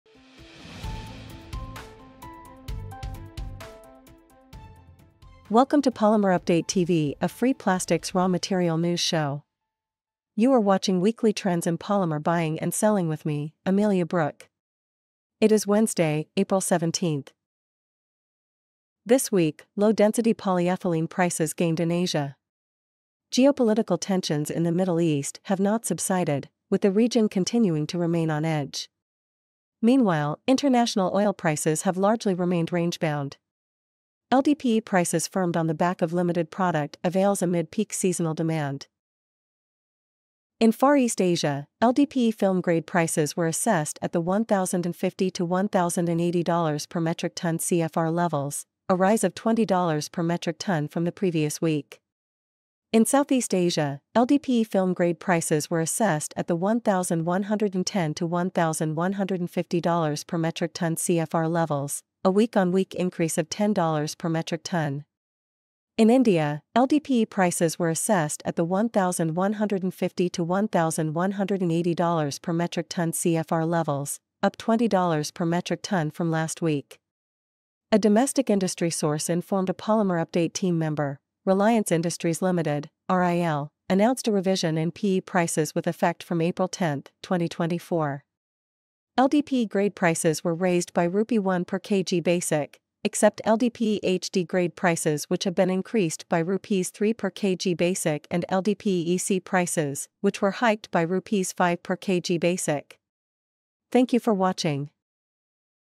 Polymer News : Low Density Polyethylene Prices Rise By $10-20/MT In Asia
Today's polymer news and price changes in the global petrochemical industry with Amelia Brook.